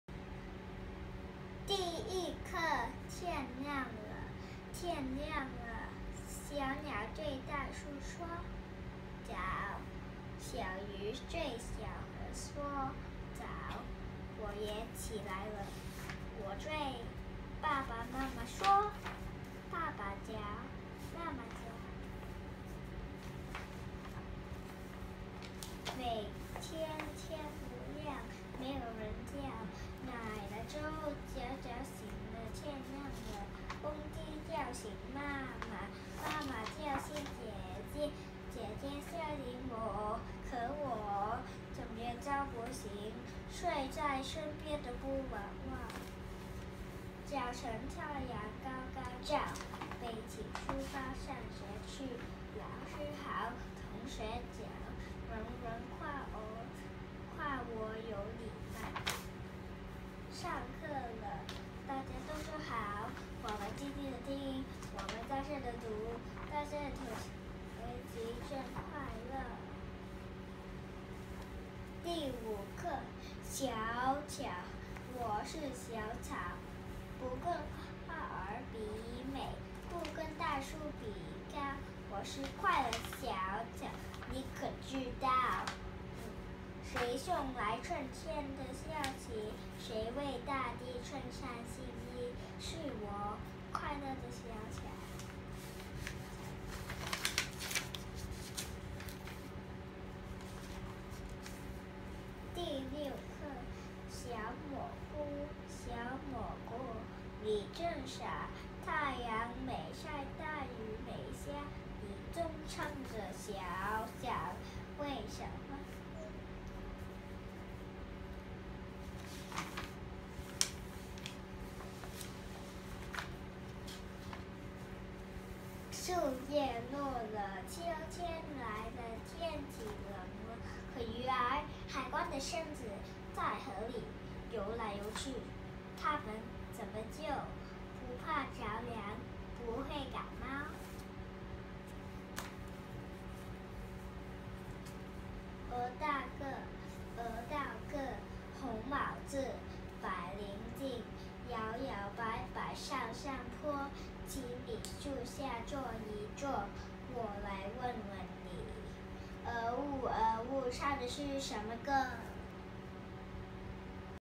Chinese
Mia is reading a Chinese book. Jnndjdhrhrhurhrhhdrbhhrhh daughter and daughter of course u to the jdjngg